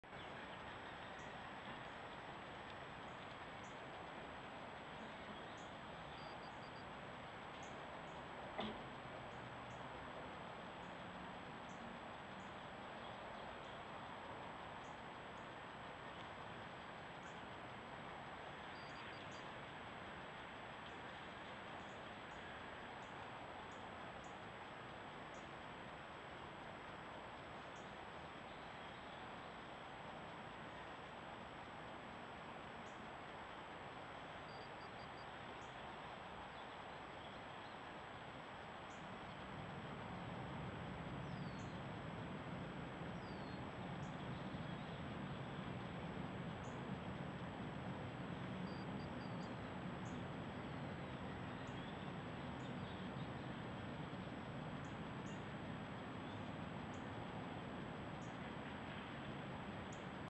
Browning Trail Cam: 2 Coyotes 7/31/24 7:41am
Two coyotes
July 31, 2024
7:41am
Garden Ridge, TX